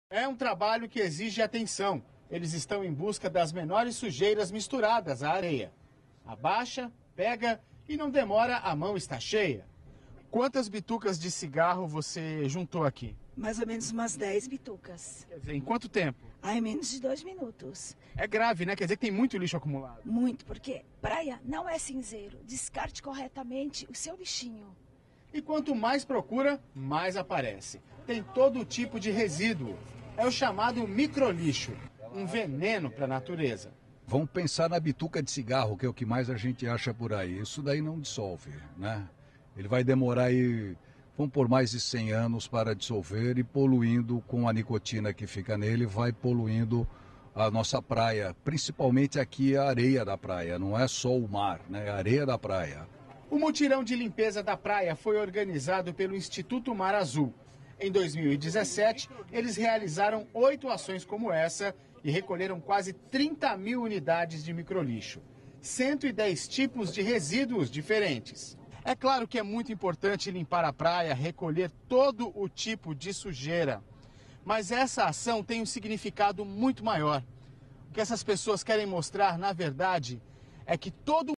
Mutirão de limpeza é realizado na praia de Santos
Um mutirão de limpeza foi realizado na praia de Santos. A concentração aconteceu perto da concha acústica. O Instituto Mar Azul recolheu cerca de 12 quilos de microlixo.